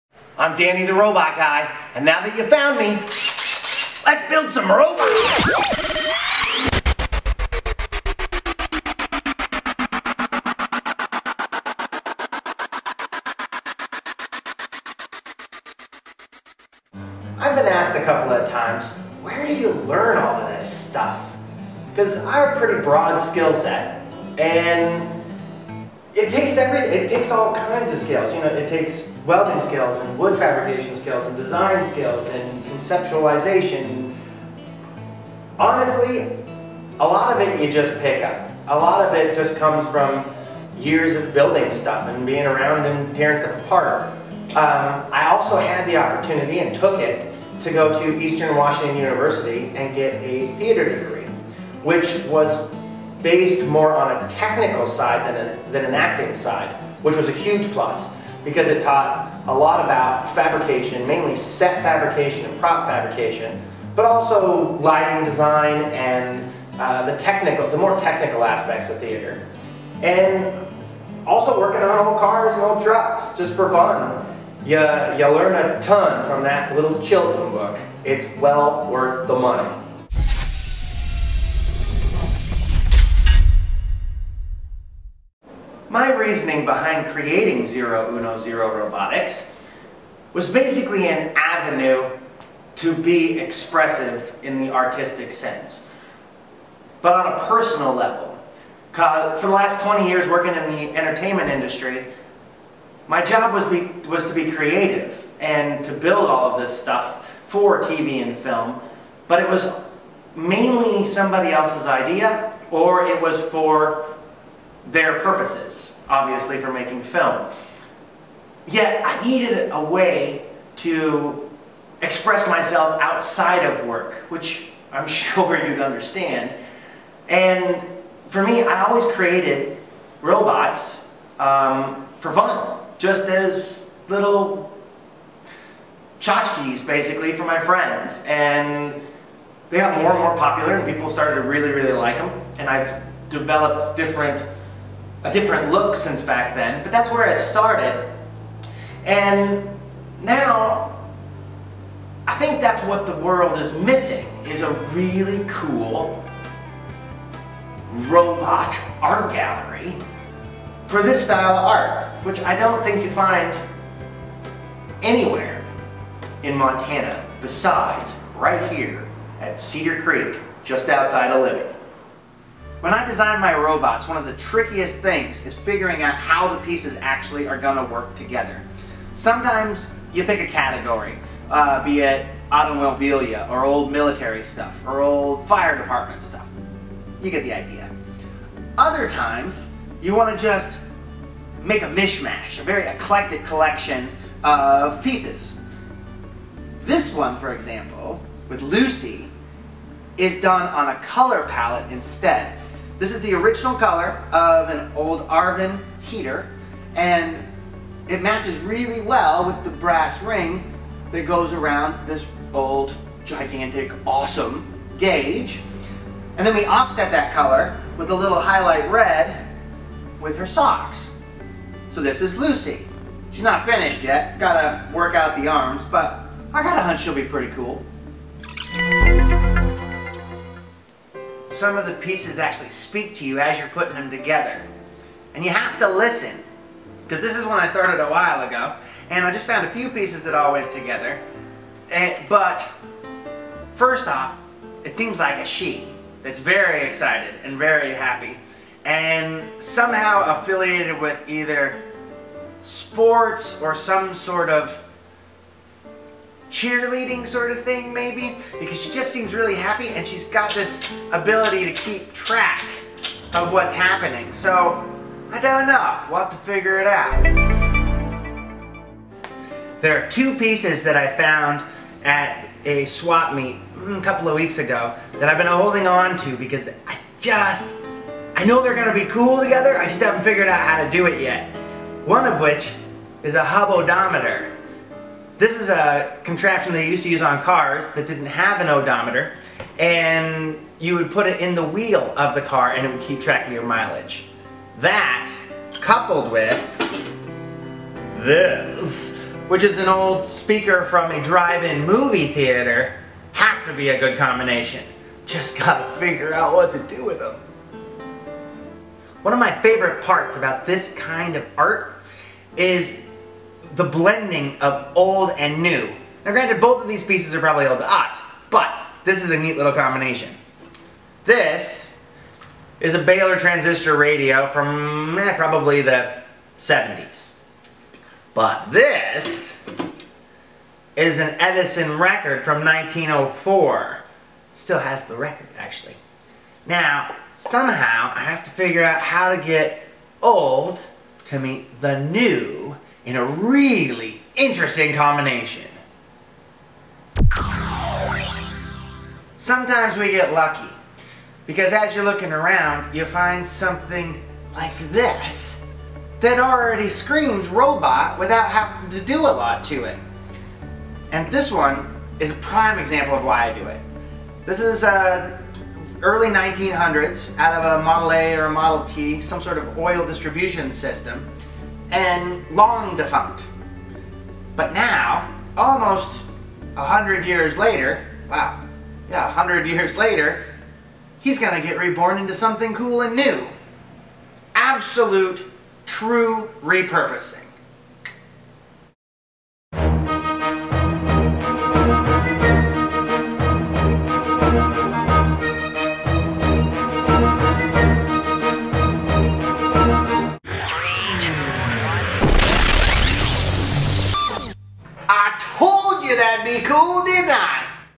Where Did Danny The Robot Guy Learn How To Build Robots . . . and Other Stuff?
Danny gets serious and tells us how he got to know so darn much about building robots and all the other stuff he builds (including movie, TV and Hollywood award show sets!). Repurposing with a purpose is what Danny The Robot Guy loves to do!